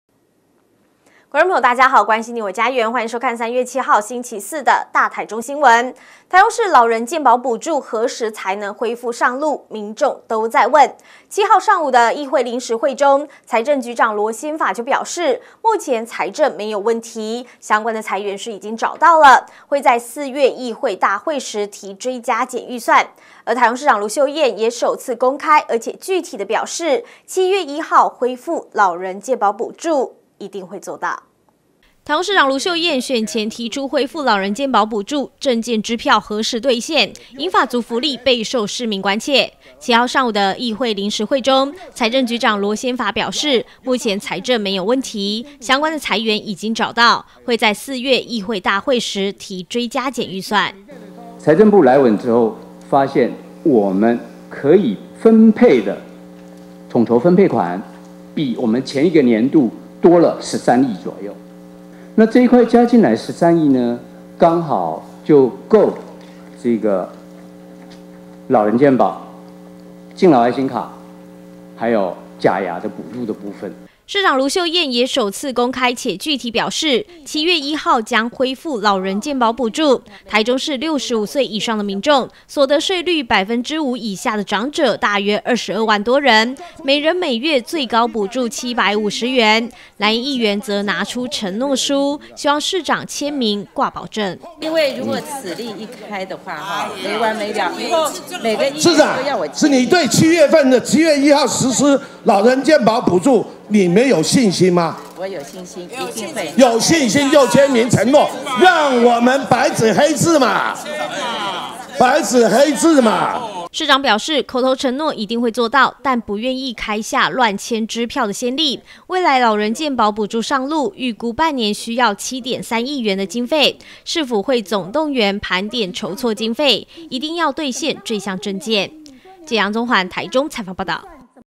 大台中新聞-7/1起恢復台中老人健保補助 盧秀燕掛保證
台中市老人健保補助何時才能恢復上路，各地民眾都在問，7號上午的臨時會中，財政局長羅仙法表示，目前財政沒問題，相關財源已經找到 ，會在四月議會大會提追加減預算 ，市長盧秀燕也首次公開且具體表示， 7月1日恢復老人健保補助！，一定會做到  。 「以上新聞可由top NEWS APP收看」